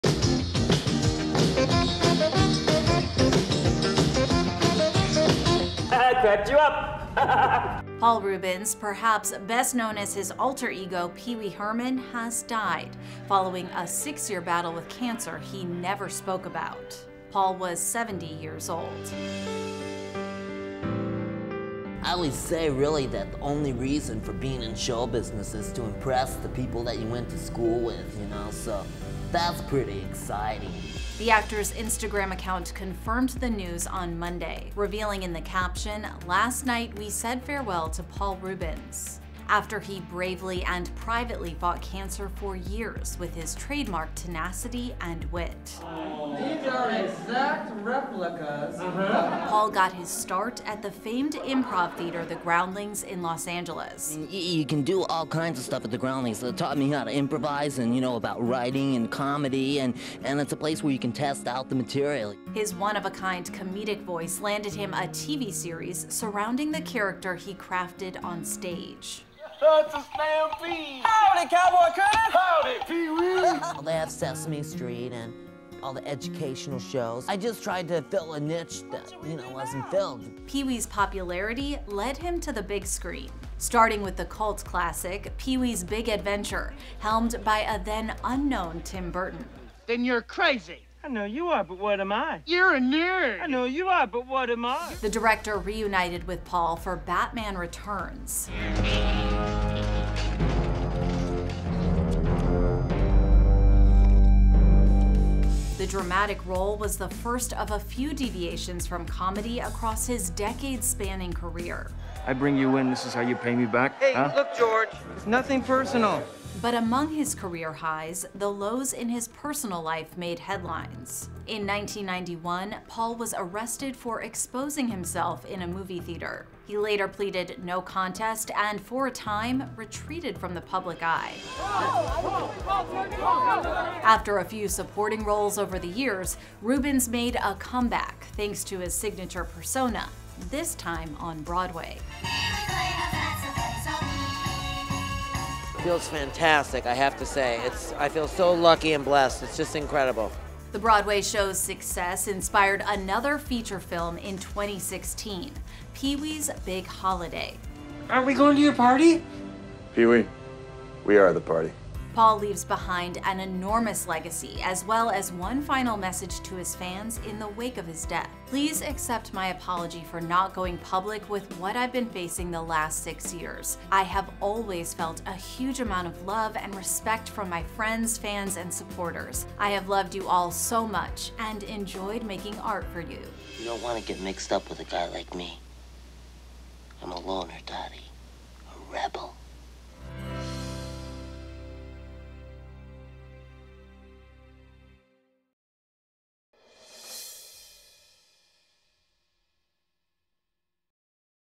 Paul Reubens, Pee-wee Herman Actor, Dead at 70
Pee-wee Herman actor Paul Reubens has died. He was 70. A statement on Paul's Instagram account confirmed the tragic news on Monday, revealing that he had privately fought a six-year battle with cancer. Alongside ‘Pee-wee’s Playhouse' and 'Pee-wee's Big Adventure,' Reubens also showed off his dramatic chops in 'Batman Returns' and 'Blow.' He later returned with his signature character for a Broadway production in 2010, followed by another feature film, ‘Pee-wee’s Big Holiday,' in 2016. In the Instagram tribute, Paul left a message for his longtime fans: 'Please accept my apology for not going public with what I’ve been facing the last six years. I have always felt a huge amount of love and respect from my friends, fans and supporters. I have loved you all so much and enjoyed making art for you.'